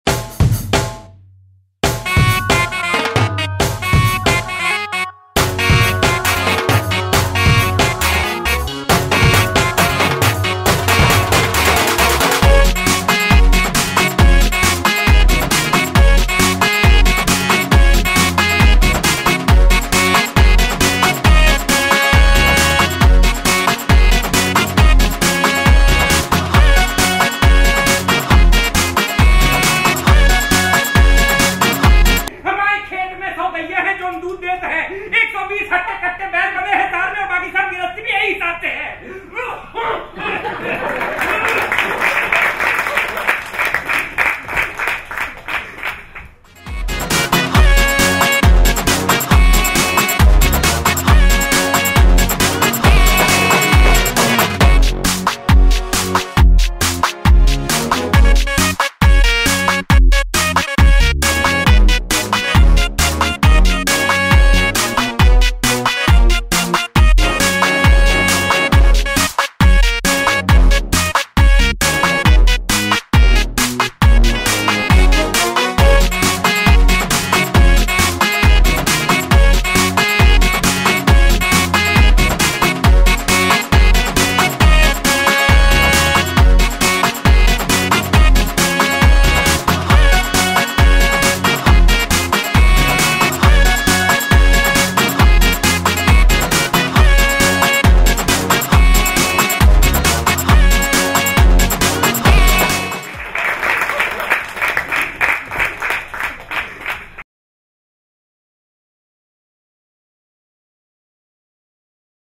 [Trailer] Taming of The Shrew By Naatak
Naatak presents Shakepeare's The Taming of the Shrew in Hindi from March 14-29, 2015 in the Tabard Theater San Jose.